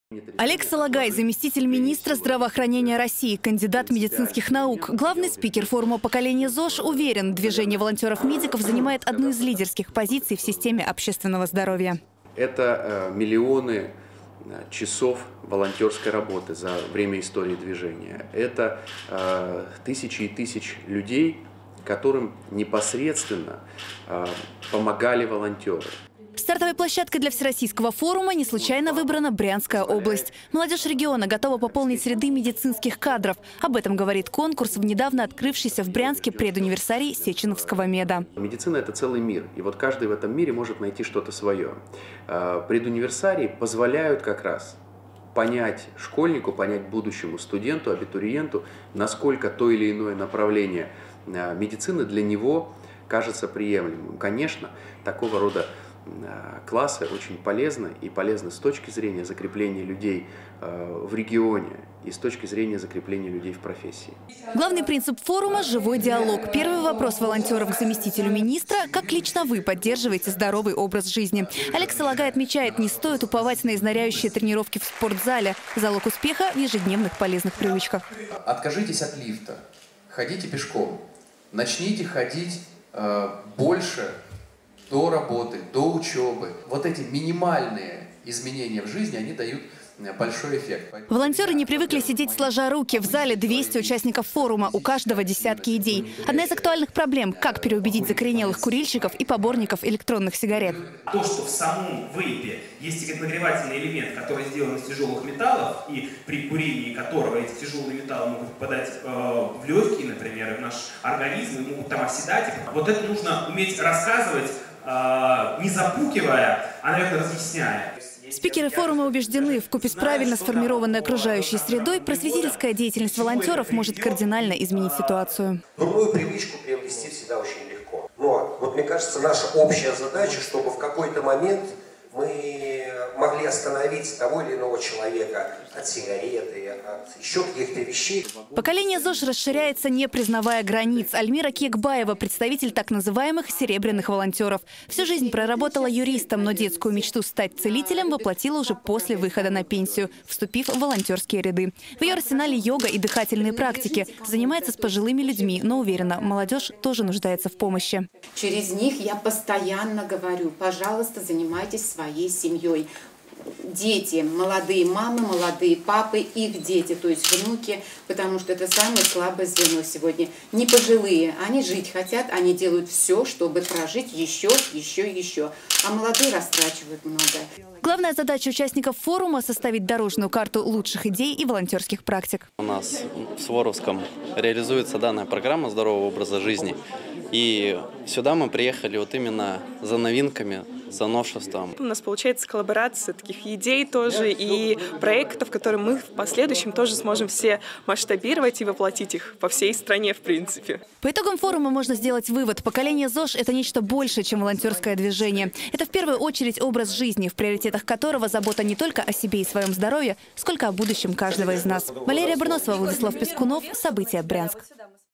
Замминистра здравоохранения России посетил форум "Поколение Zож" в Брянске 25 11 19
Заместитель министра здравоохранения России Олег Салагай посетил Брянск, чтобы пообщаться с участниками Всероссийского форума волонтёров-медиков "Поколение Zож". Главная тема конвенции - популяризация здорового образа жизни.